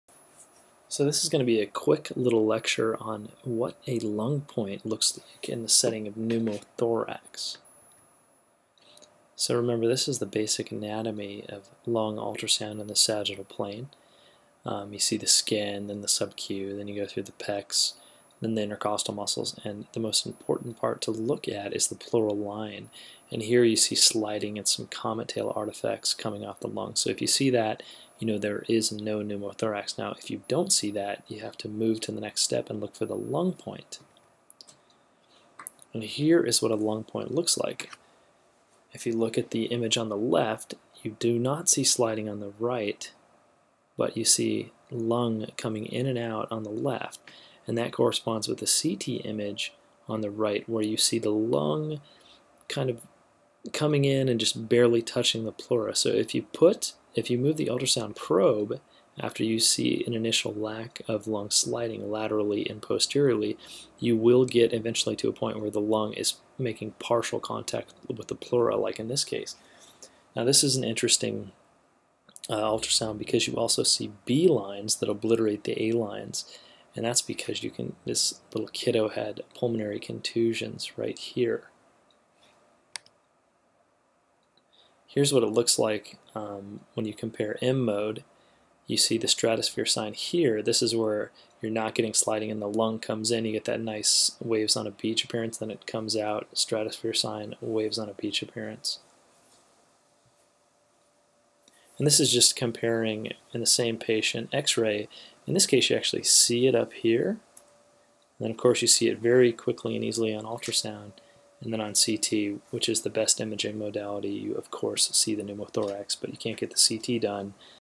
Lung Ultrasound Pneumothorax with a Lung Point
2 minute video showing an example of a lung point in the setting of a pneumothorax caused by trauma.